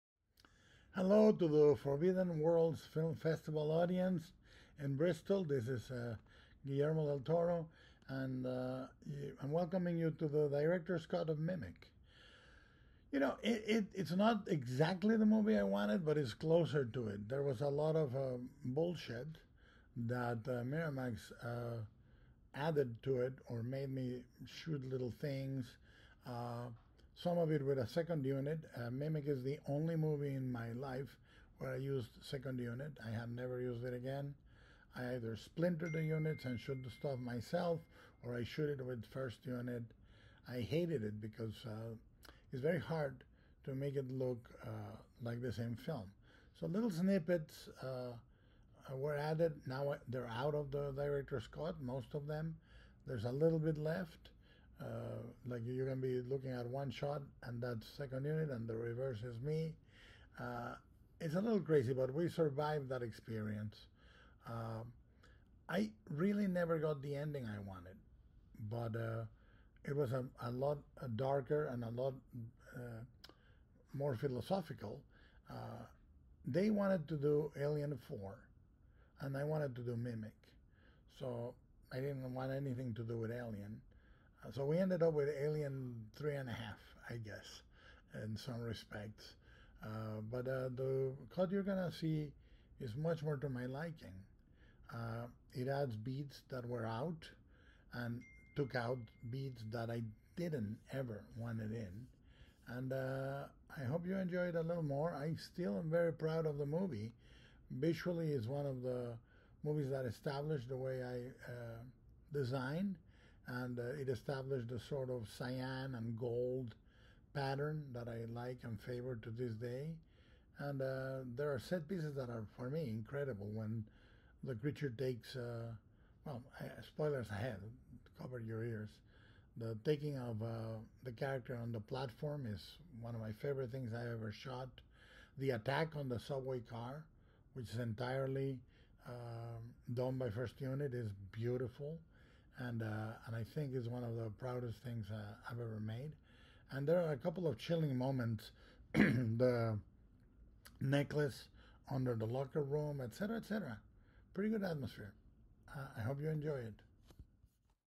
Guillermo Del Toro reflects on Mimic: The Director's Cut | Forbidden Worlds - The Big Scream 2023
Having won deserved critical acclaim for his feature debut, Cronos, Mexican filmmaker Guillermo del Toro popped over to Hollywood for his next film. Boasting a cast list that includes Mira Sorvino, Josh Brolin and F. Murray Abraham, and set about making a gloopy giant-bug movie. Initially released in a version overseen by a notorious producer who we shall not name, del Toro was finally able to complete his Director’s Cut in 2010 – and that is the version that we shall be presenting.

The Oscar-wining director was kind enough to send us this special intro (on the week of his birthday) ahead of the screening.